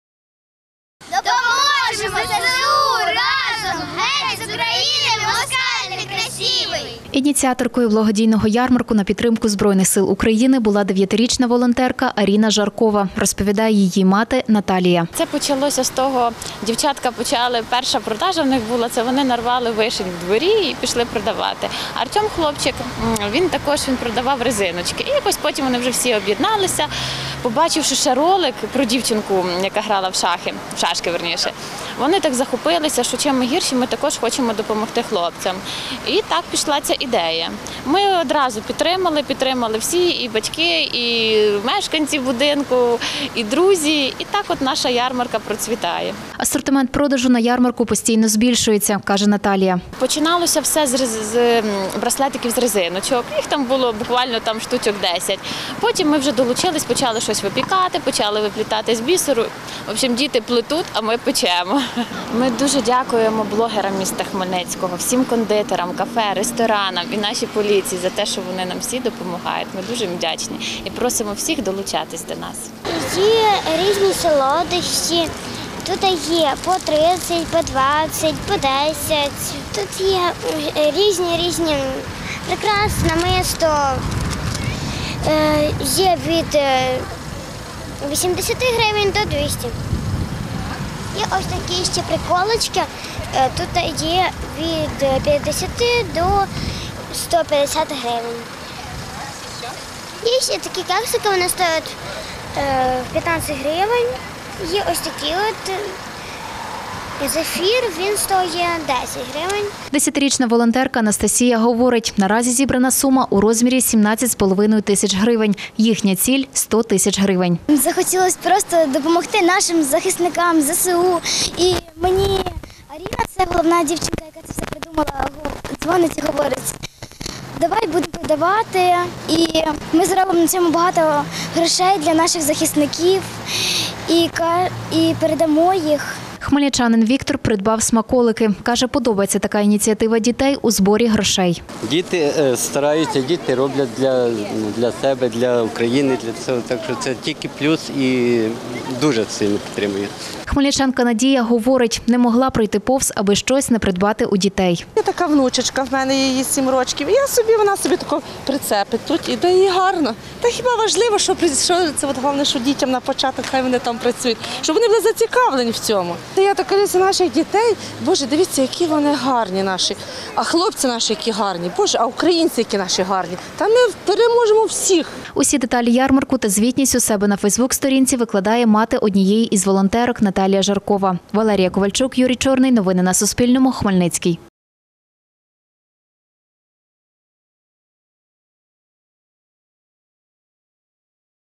У Хмельницькому юні волонтери влаштували благодійний ярмарок
Смаколики, домашню випічку та саморобні прикраси продають на благодійному ярмарку юні волонтери з Хмельницького. Розповіла Суспільному дев’ятирічна  ініціаторка ярмарку Аріна Жаркова. Каже, влаштували ярмарок, на підтримку Збройних Сил України. Ціль збору - 100 тисяч гривень. Скільки наразі уже зібрано грошей та як реагують жителі міста – далі у сюжеті.